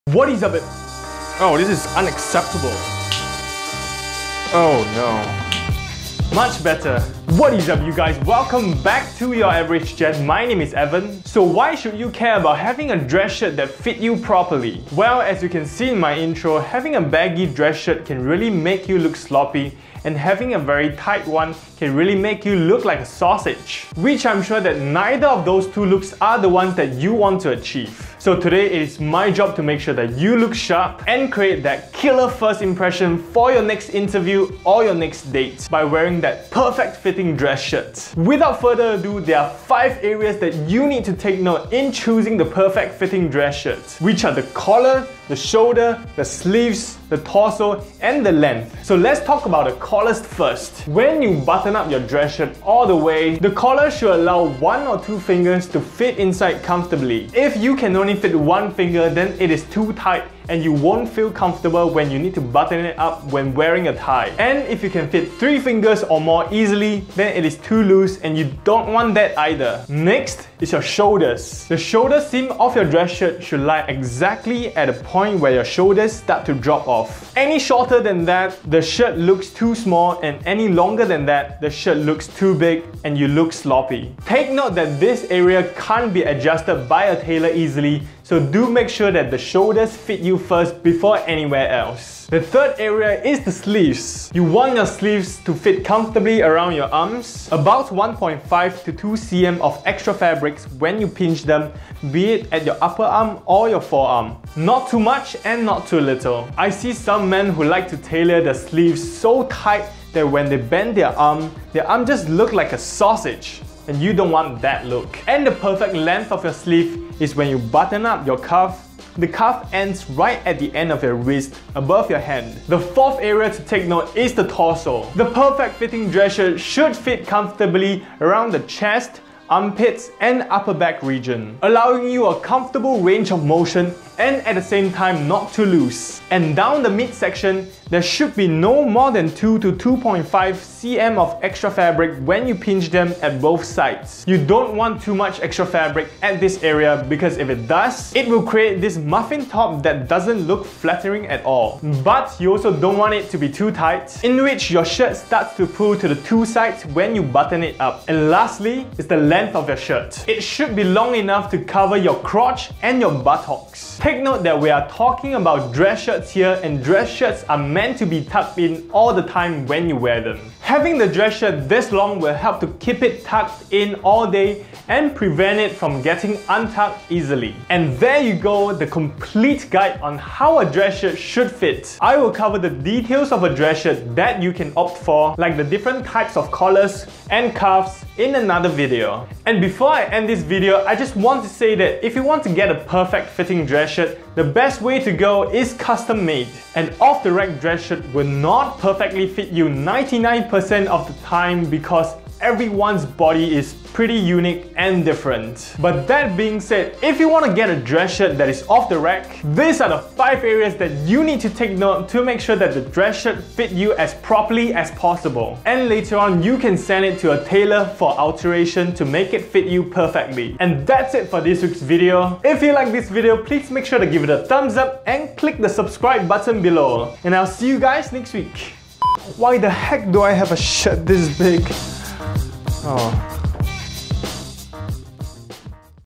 How a Dress Shirt SHOULD FIT
Dress Shirt, a piece of garment most men will wear to work or to a fancy dinner date. Does your dress shirt fit you? Check it out in this video!